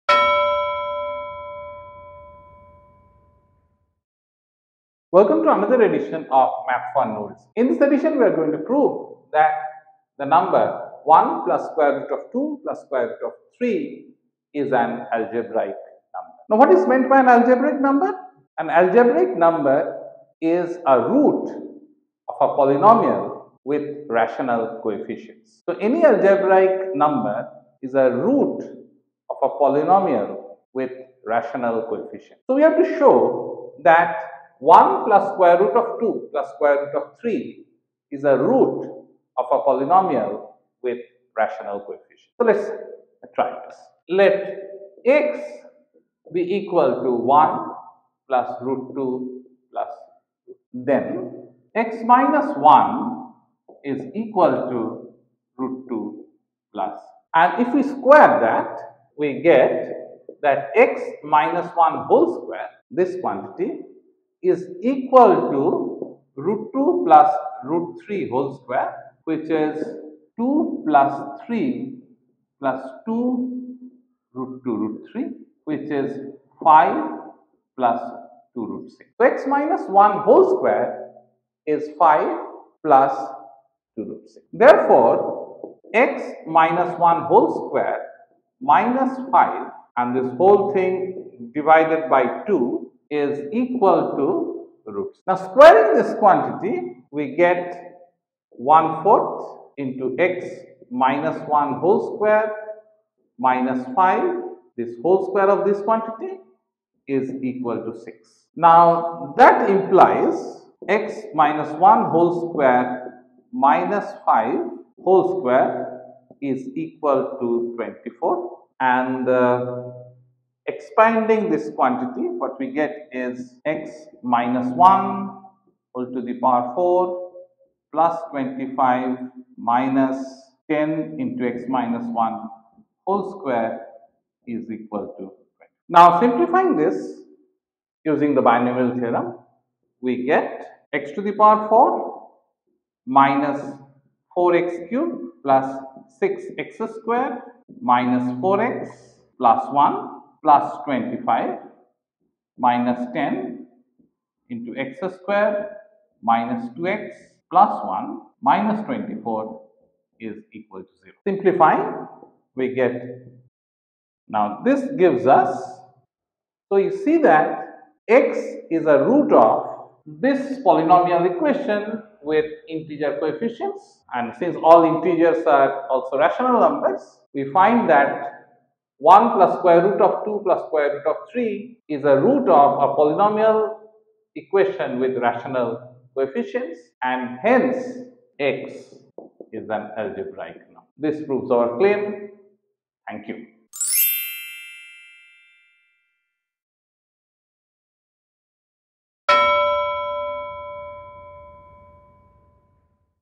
Math for No Reason 82
In this video, we show that a given number is algebraic.

A number is algebraic if it is a root of a polynomial with rational coefficients. All rational numbers are algebraic and some irrational numbers are algebraic too. However, all irrational numbers are not algebraic. Prominent examples being the irrational numbers e and pi.